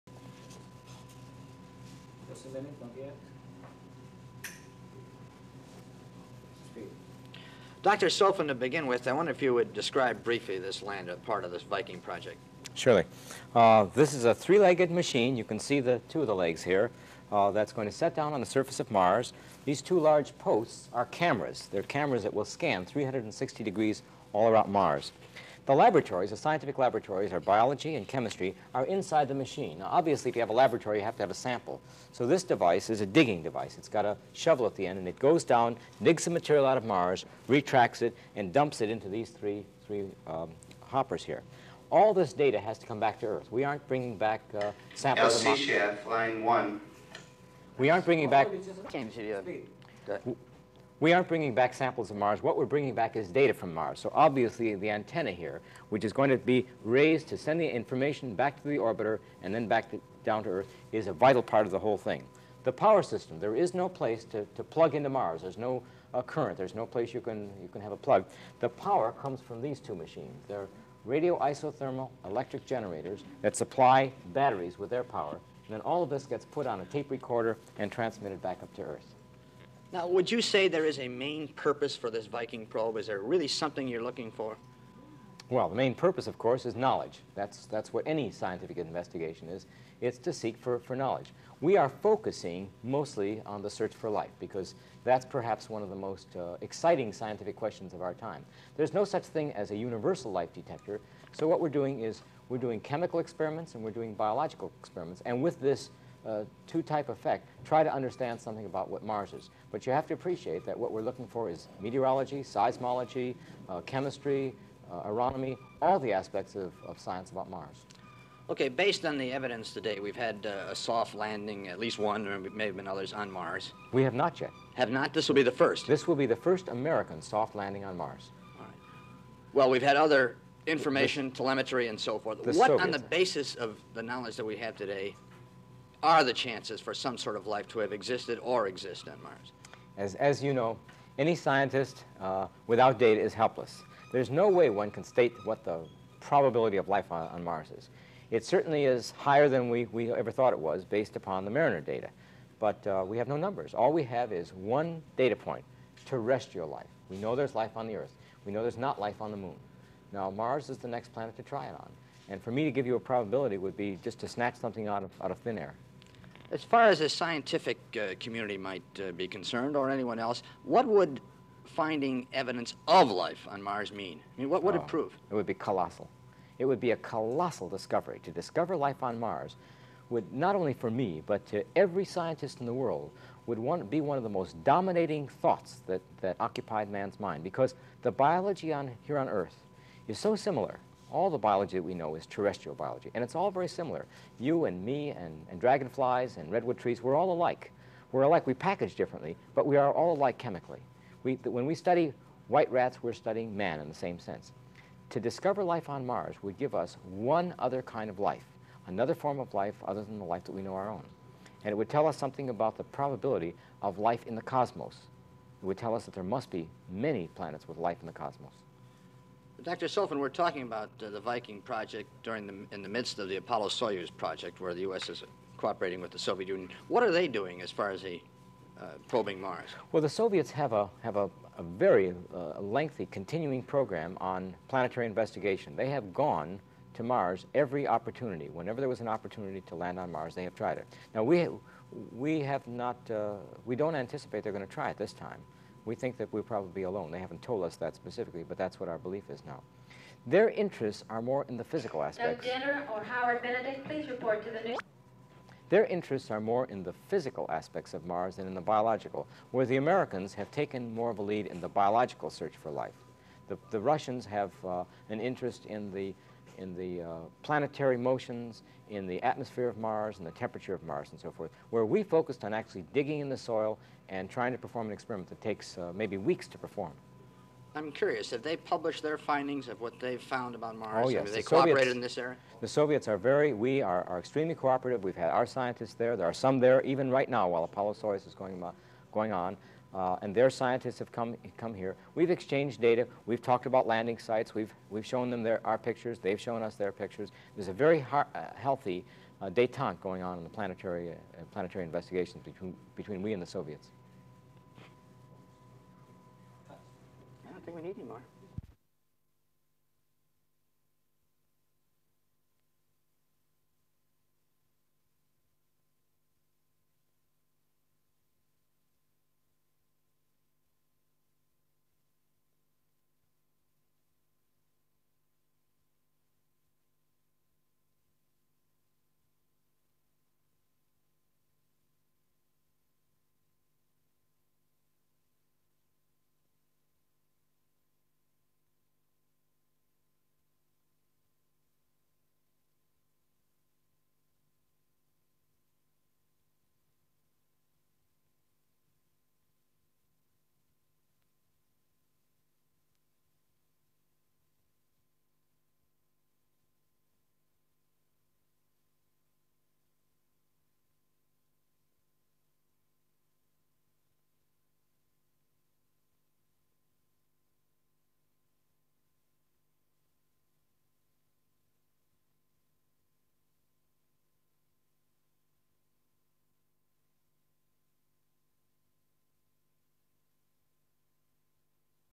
UPITN 4 9 75 INTERVIEW WITH DR SOFFEN ON VIKING MISISON TO MARS
(4 Sep 1975) At the Kennedy Space Centre in Florida Dr Soffen, the Viking Project Scientist, gave a breifing to the press regarding the Viking mission to Mars.